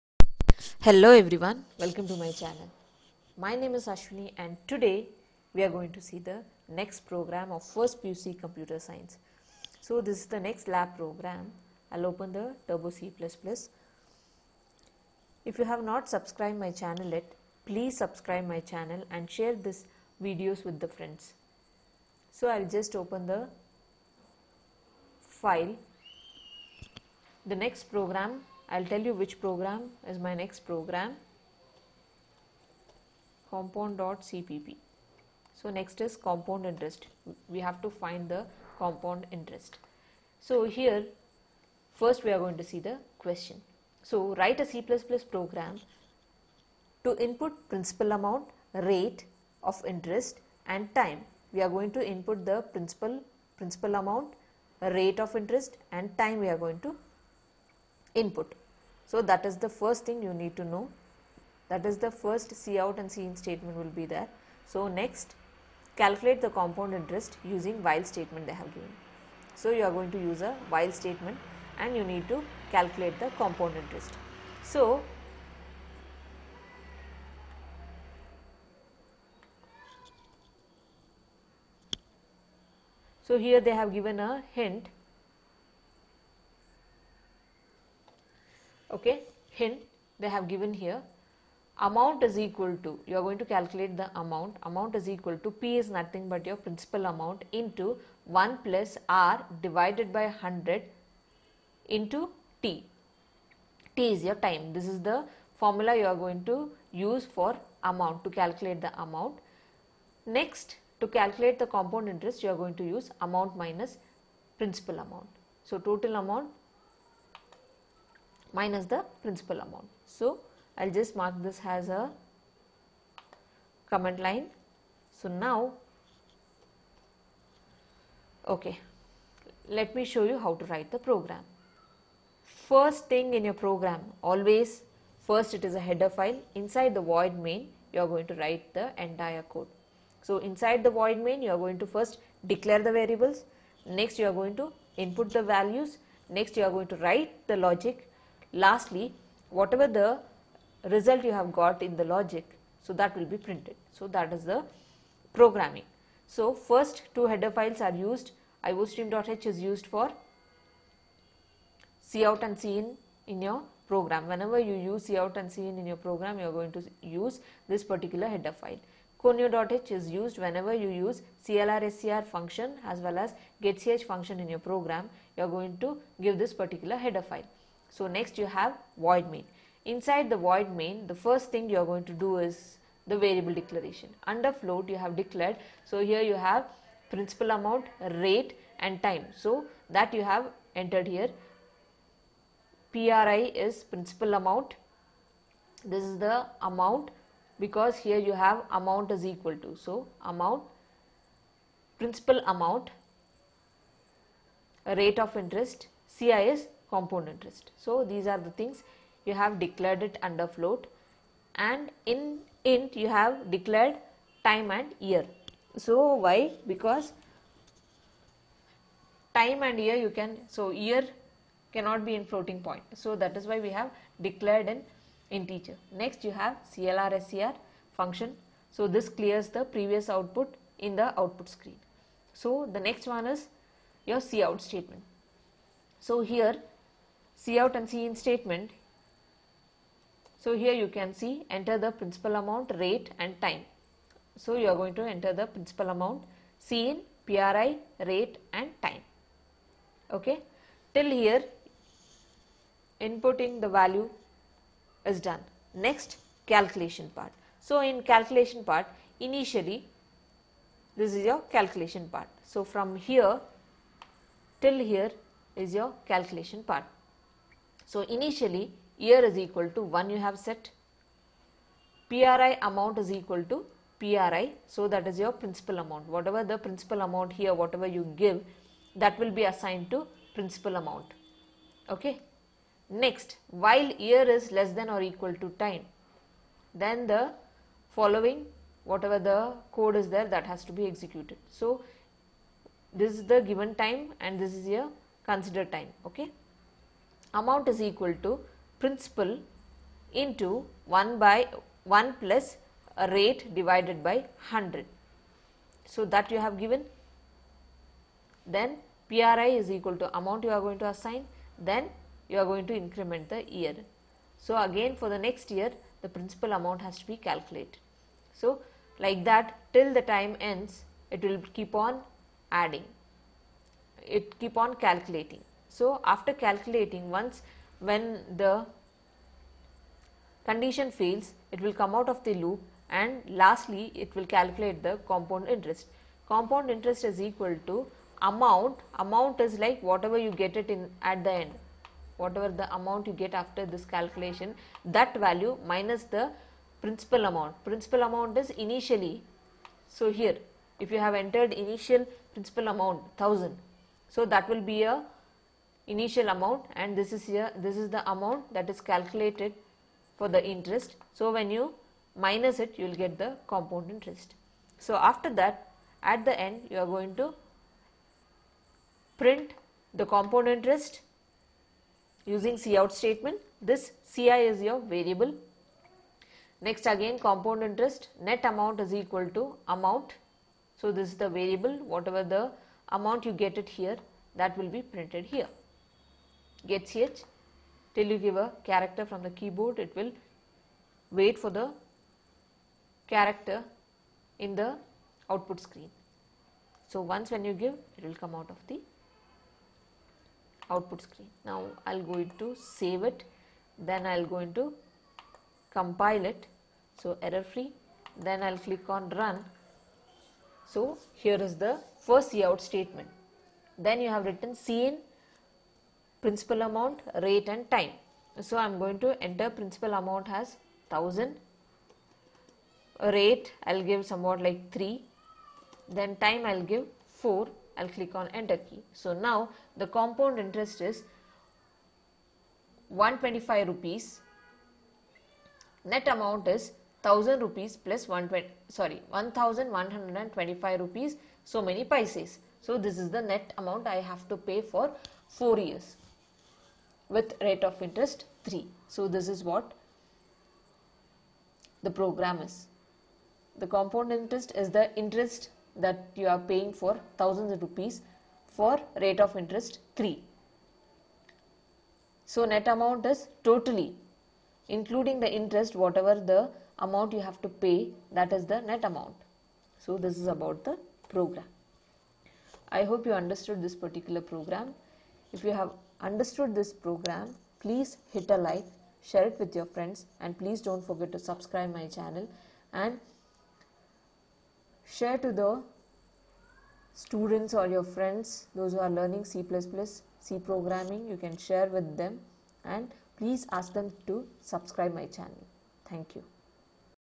Write a c++ program to input principal,rate and time Calculate compound interest use while statement
Write a C++ program to input principal amount, rate of interest and time
period. Calculate compound interest using while statement.